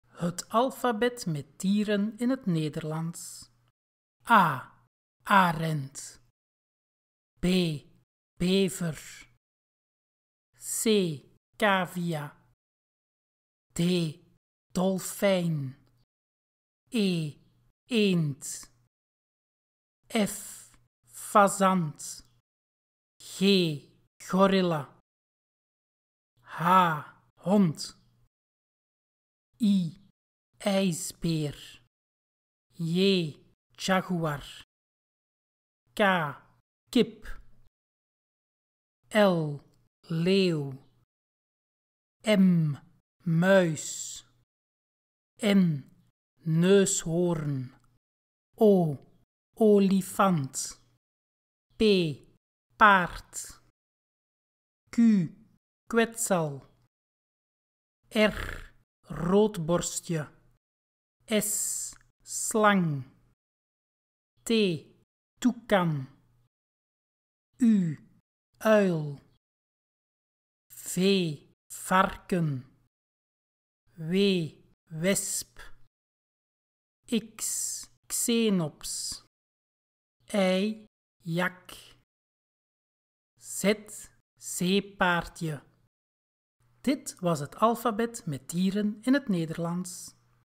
Het Alfabet met Dieren in het Nederlands | Nederlands voor Kinderen
Het Alfabet met Dieren in het Nederlands.
Luister en Leer vandaag nog het Nederlandstalig alfabet.
We leren samen de juiste klanken van alle letters van het Alfabet in het Nederlands